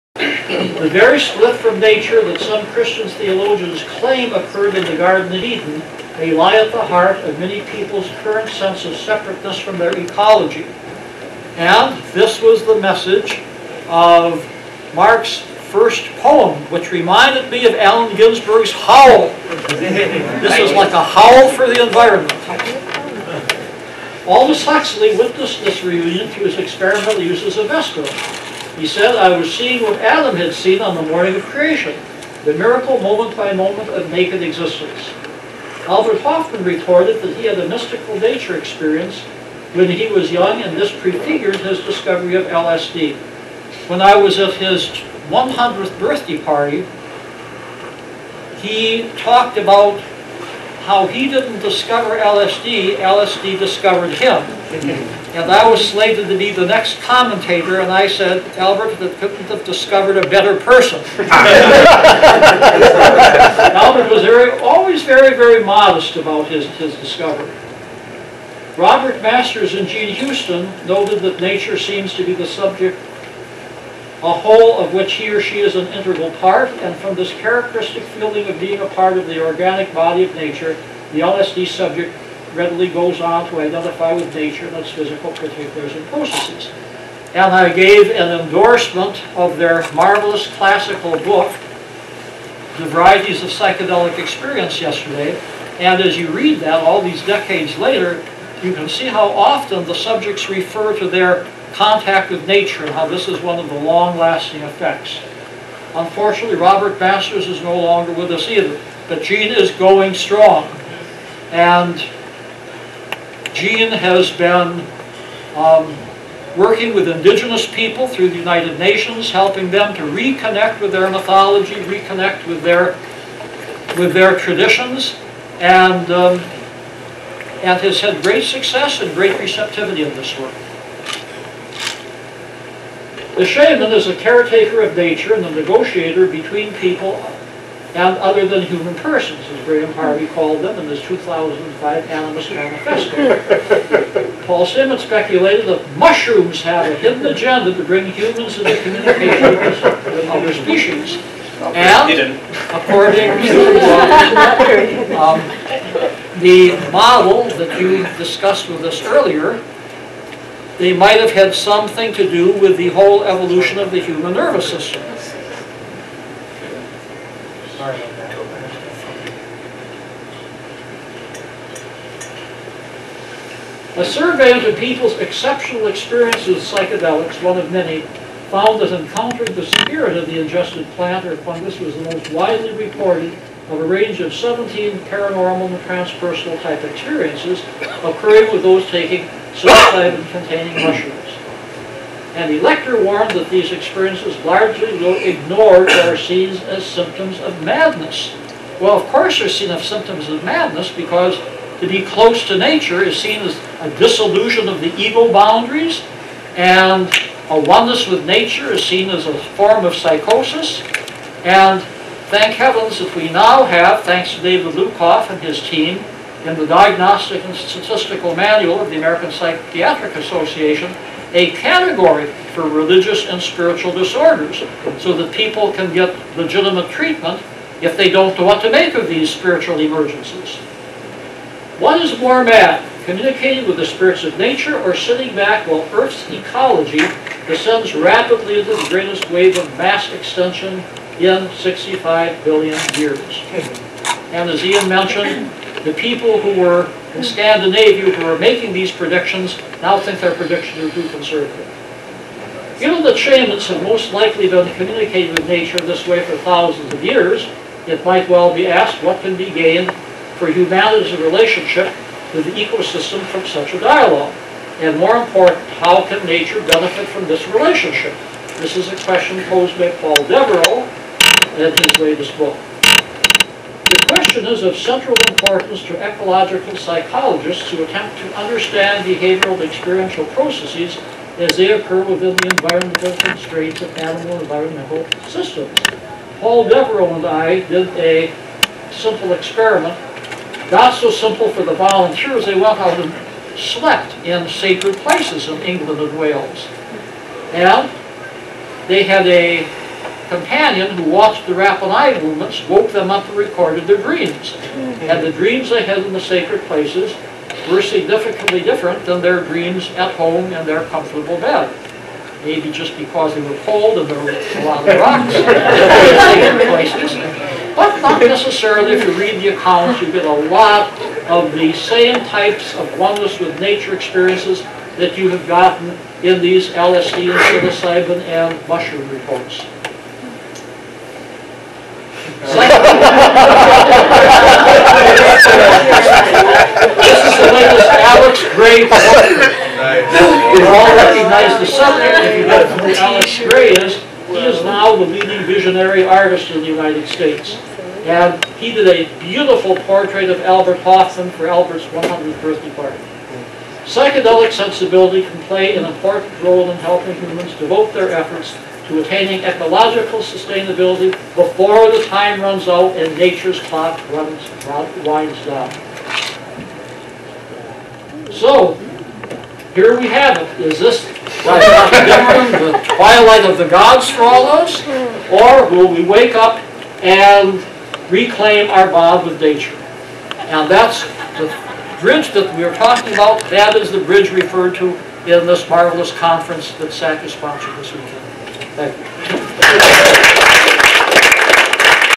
Stanley Krippner "Practices and Technologies Toward Remembering" @ SAC Conference 2008
Society For the Anthropology of Consciousness Conference 2008
Portland, Oregon

Stanley Krippner is one of the revered elders of the transpersonal psychology movement. A longtime personal friend of Albert Hoffman, inventor of LSD, Krippner has published dozens of books and articles, and edited numerous journals on dream psychology, mythology, and the investigation of paranormal phenomena. He is here presenting some of his thoughts on our "split" from nature, the human discovery of lsd, indigenous psychologies, and the prospects of some trends in contemporary politics.